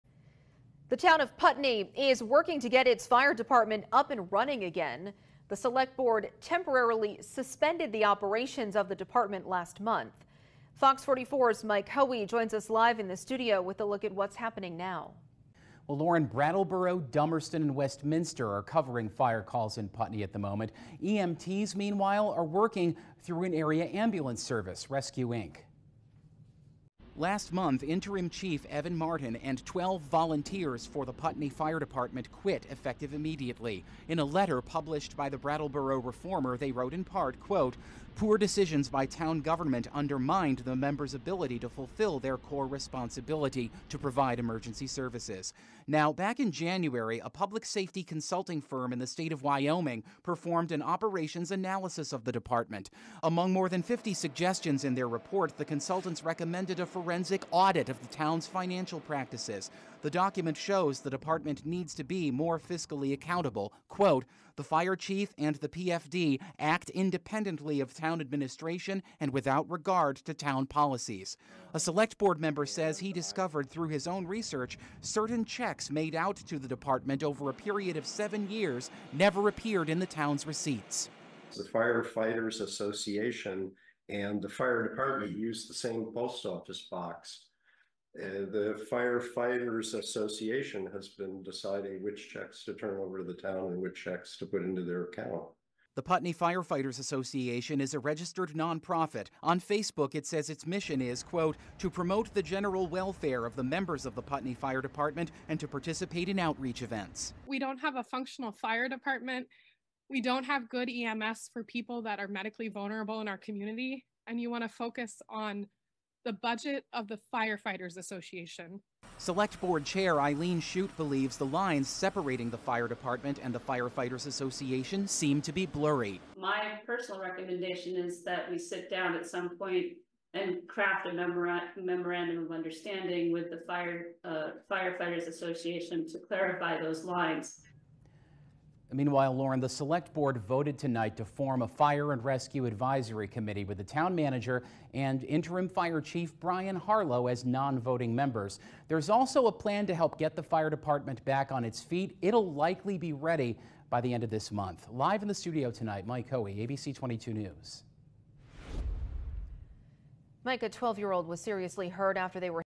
Putney works to get fire department back on its feet
The Selectboard suspended the fire department's operations last month after a wave of mass resignations.